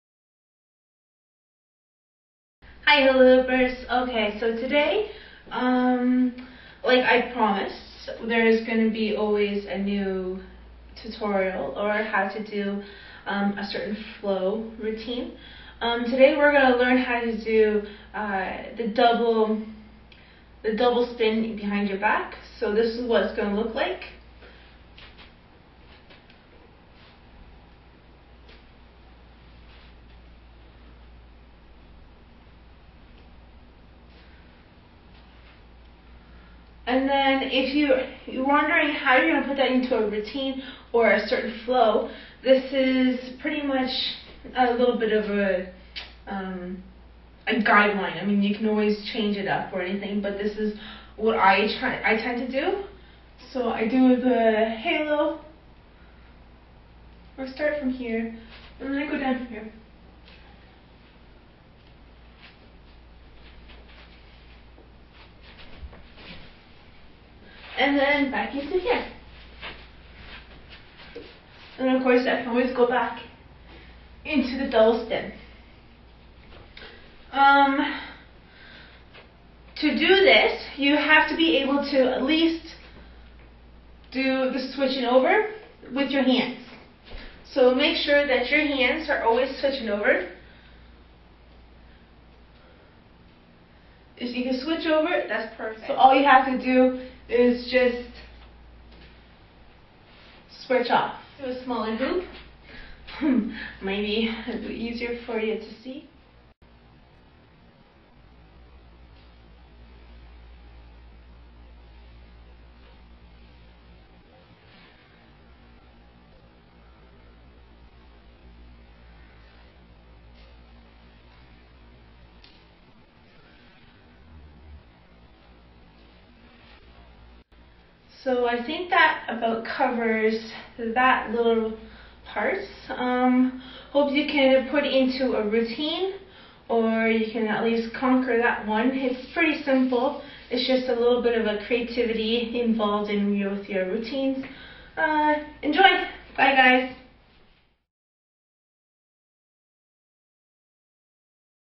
Hula Hoop Tutorial - Double Spin at Back
This is a hula hoop trick for people that want a little more creativity to their performance.